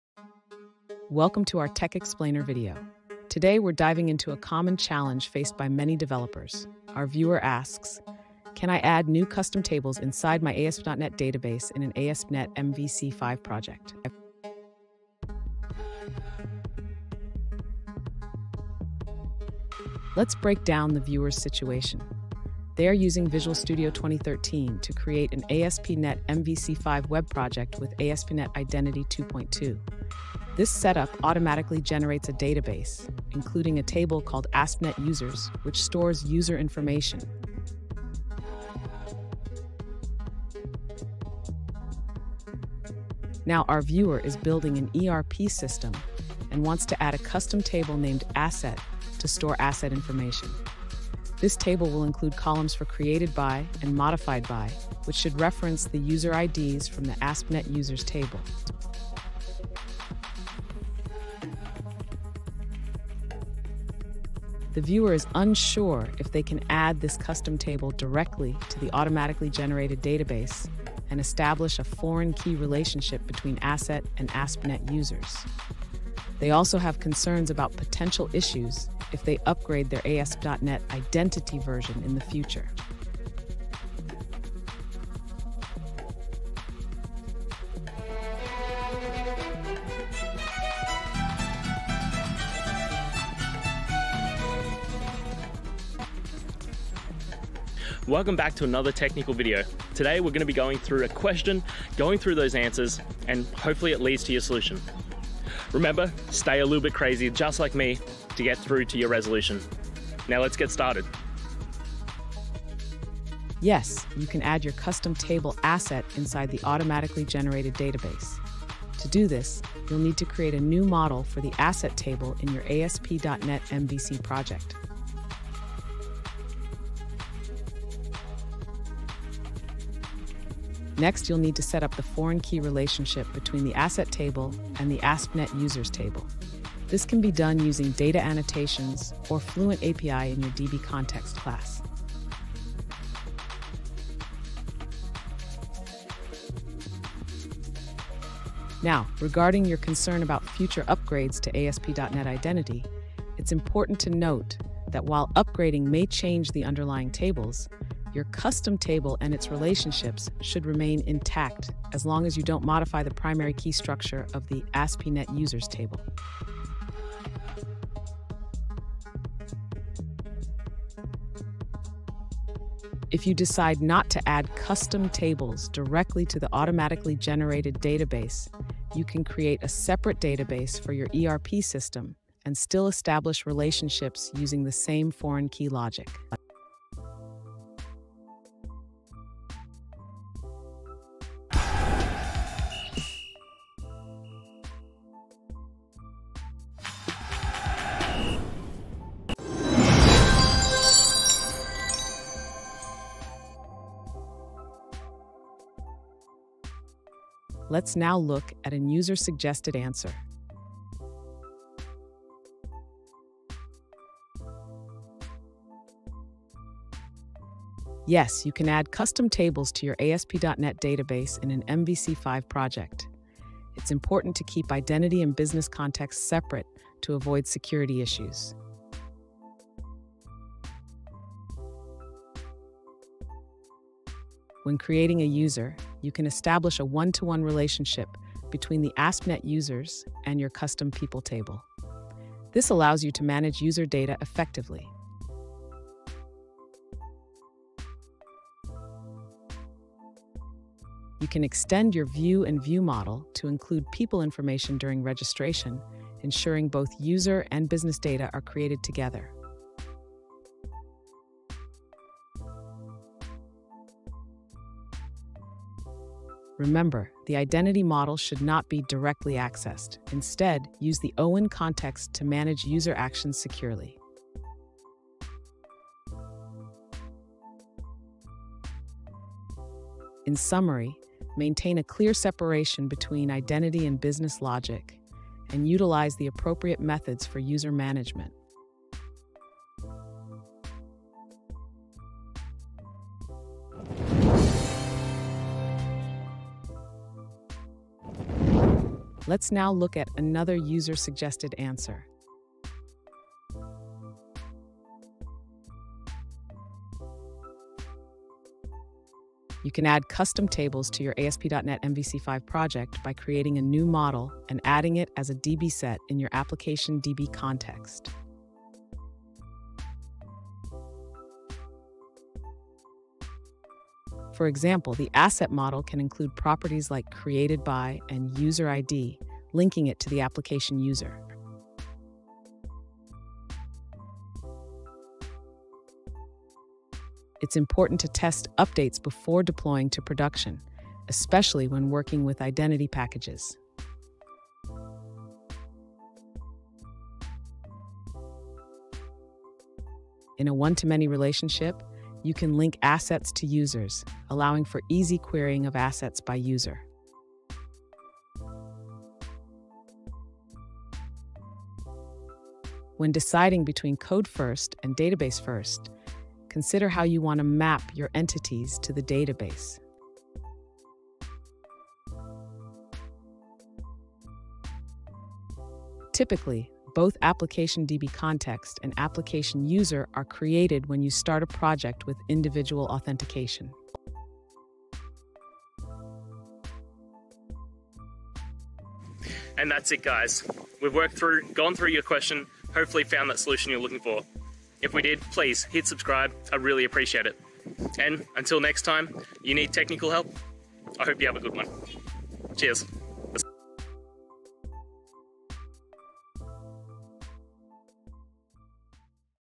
How to Add Custom Tables to ASP.NET MVC 5 Database: A Step-by-Step Guide
In this video, we will explore the process of adding custom tables to an ASP.NET MVC 5 database. Whether you're looking to enhance your application's functionality or tailor your data structure to better fit your needs, this step-by-step guide will walk you through the essential techniques and best practices. Join us as we dive into the world of database management within the ASP.NET framework, ensuring you have the tools and knowledge to create a robust and efficient application.

Today's Topic: How to Add Custom Tables to ASP.NET MVC 5 Database: A Step-by-Step Guide

Related to: asp.netmvc5, customtables, database, step-by-stepguide,